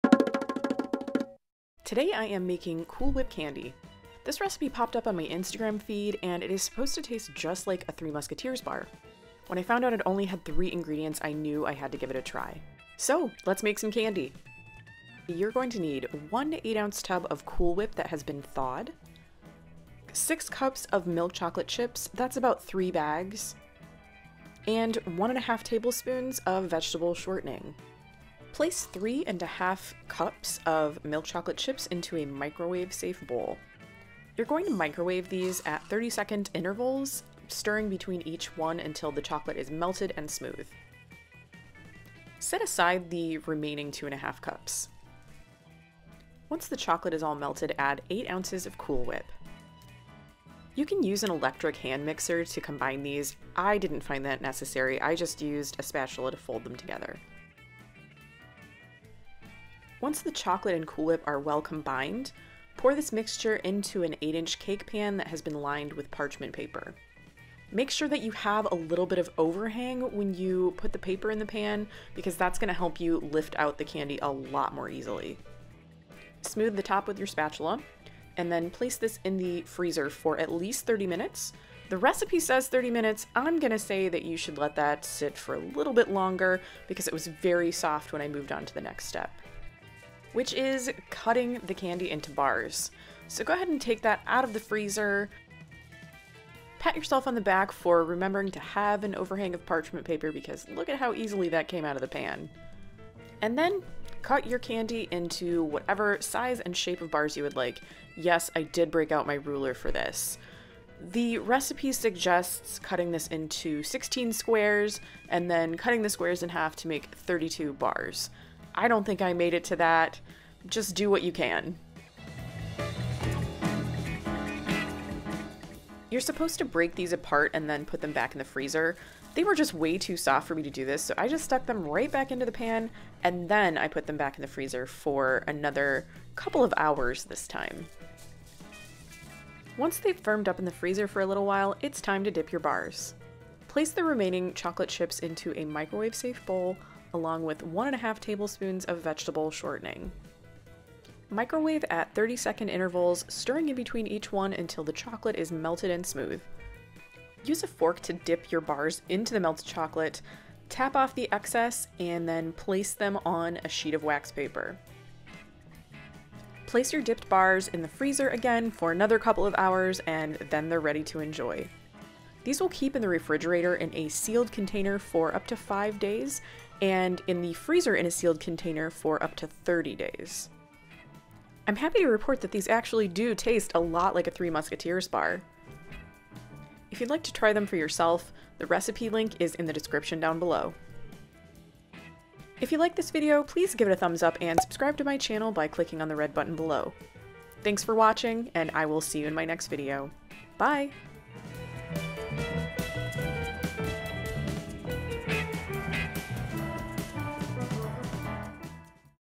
Cool Whip Candy - only 3 ingredients!! EASY AND DELICIOUS!
I'm making Cool Whip Candy that tastes just like a 3 Musketeers bar!

Items from this video:
Square cake pan
Gridded flexible plastic ruler
4qt tempered glass bowl

CAMERA GEAR:
- shooting grip/tripod